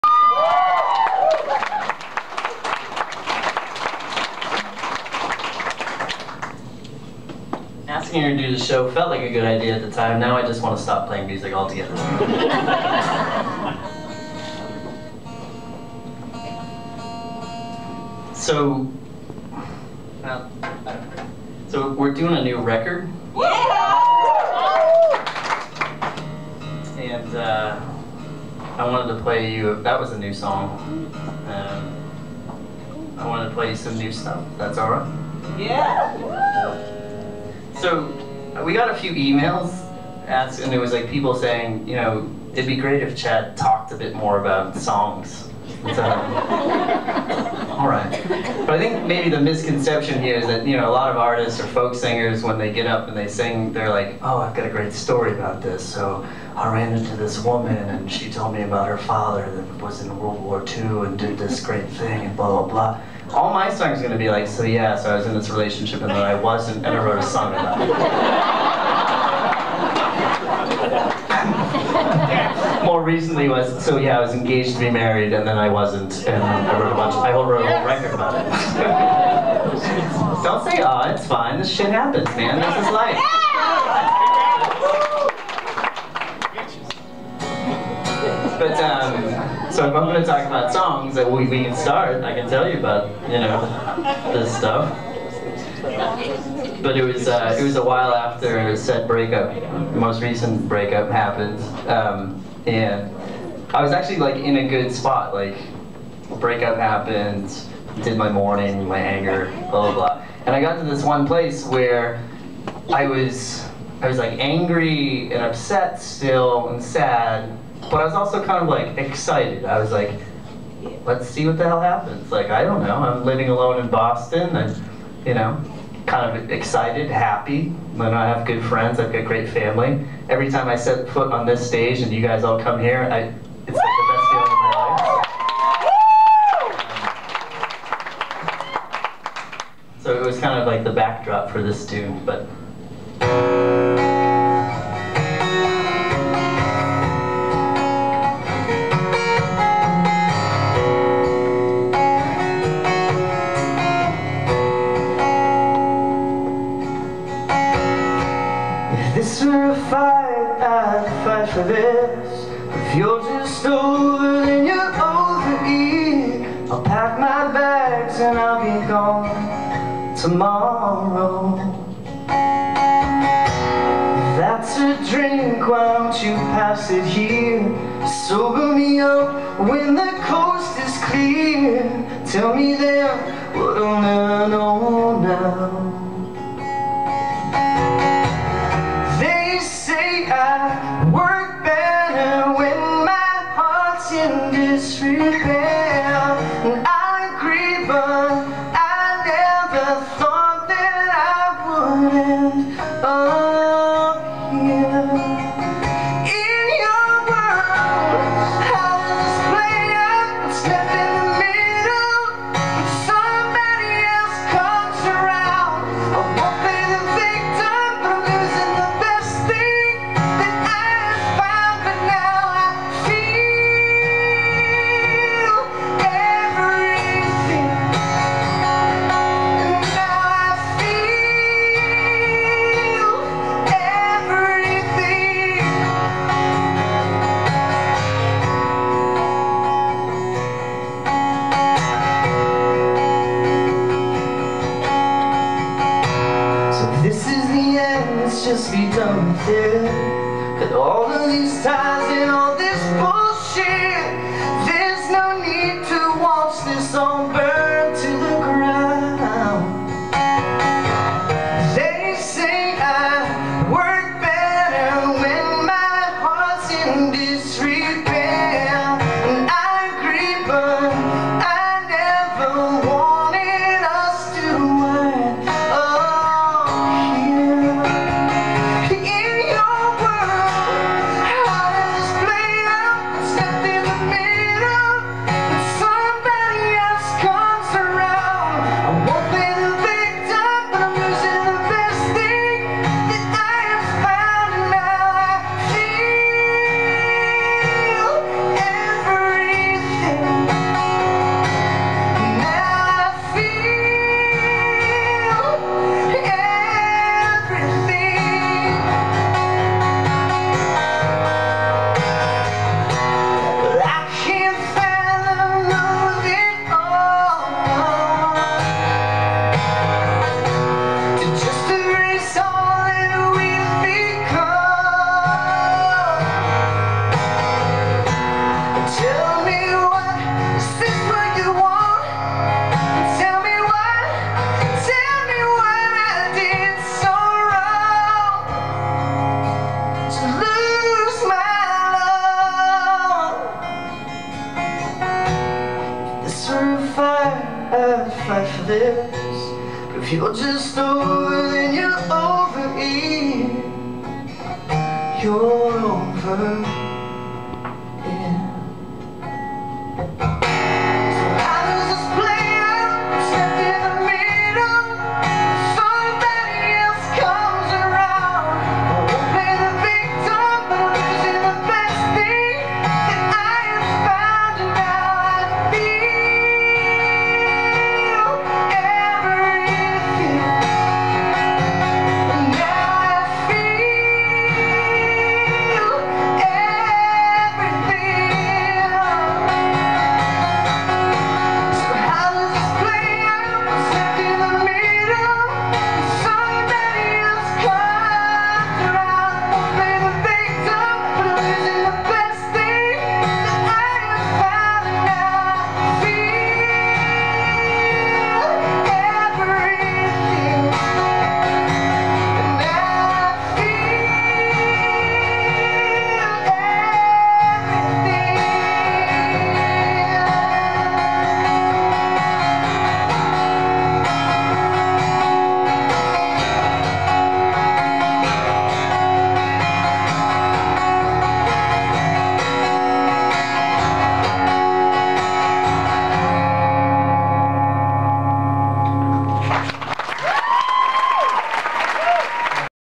Chad Tupelo May 23 2014 New Song
This is a new song from one of my favorite singer/songwriters, Chad Perrone.